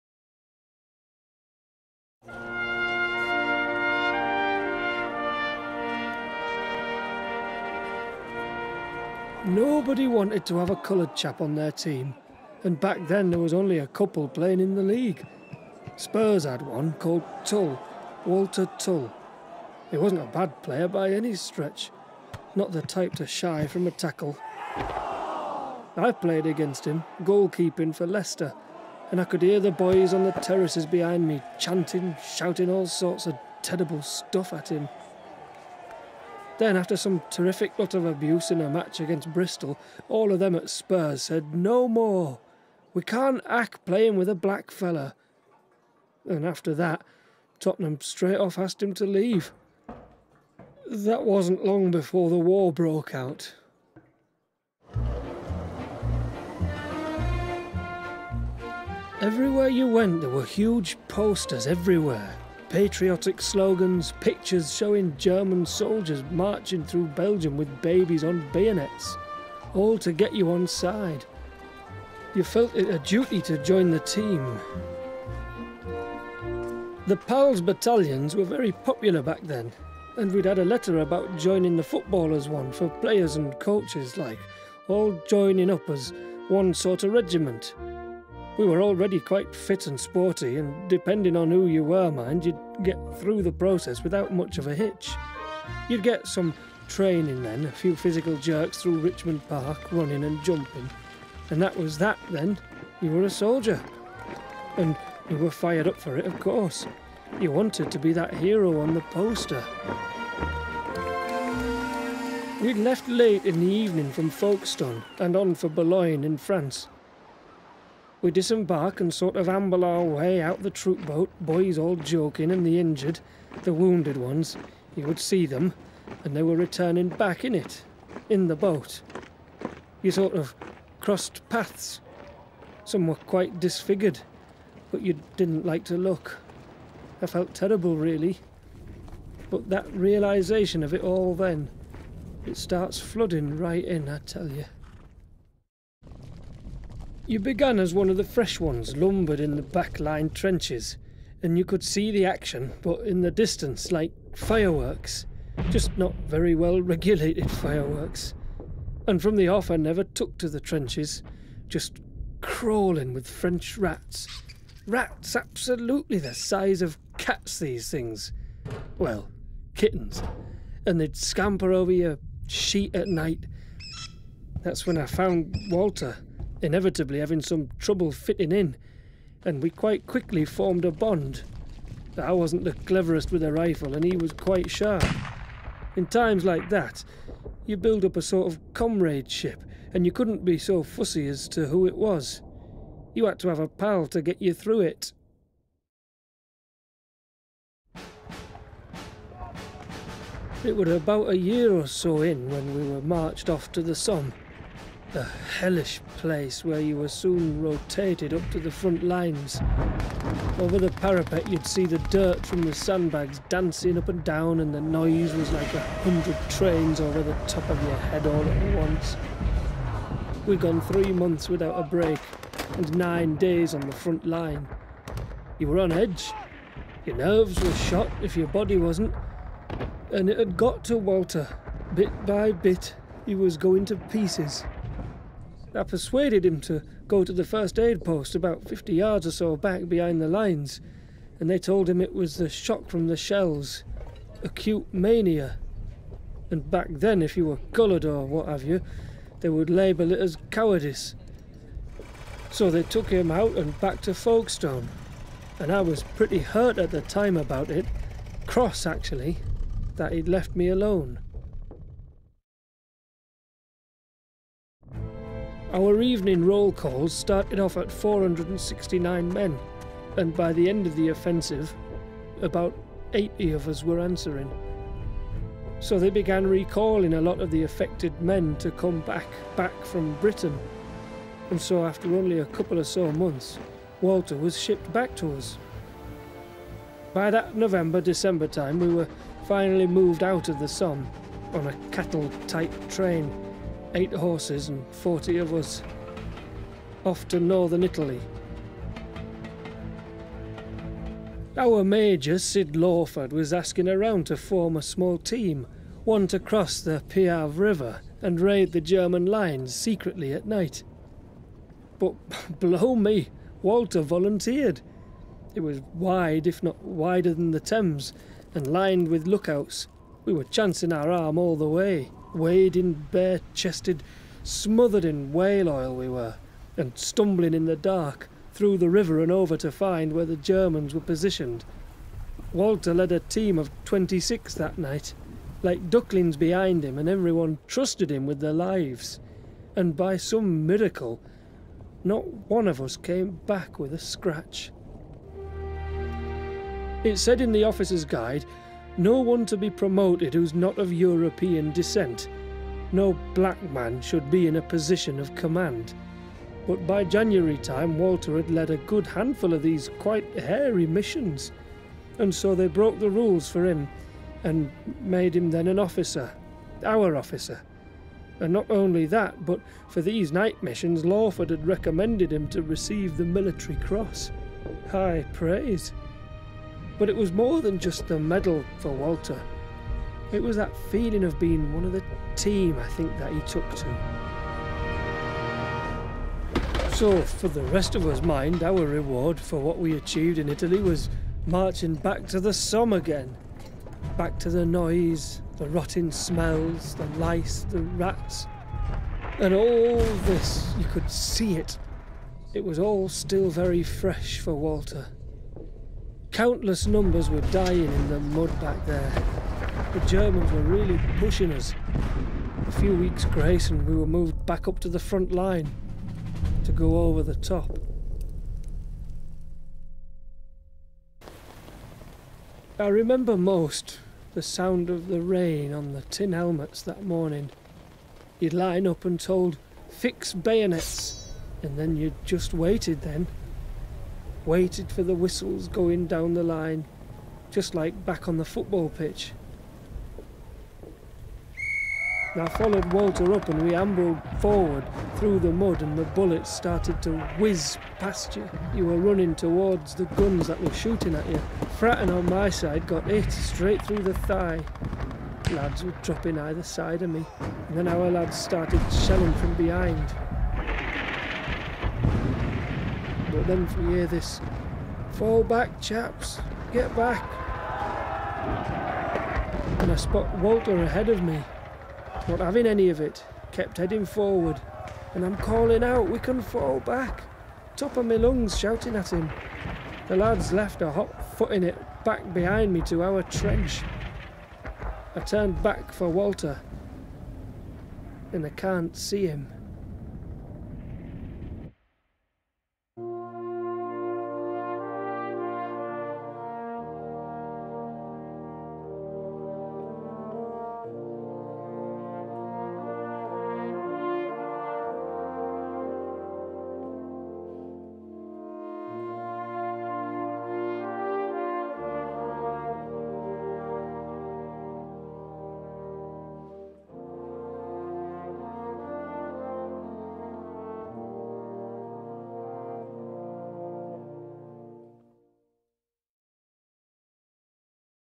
Walter Tull: Britain's First Black Officer
BAFTA nominated animation, included on the national curriculum as part of 'Black History Month' celebrating the life of Walter Tull